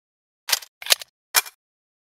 [SFM] Scar-H reload animation!
This is one of my different videos I will make, and I thought why not make a reload animation? I was inspired by the reload animation channels all across youtube, for example a good one is Hyper, great animator and he even animated the weapons in Titanfall 2 and MW2019!
Anyways Enjoy :3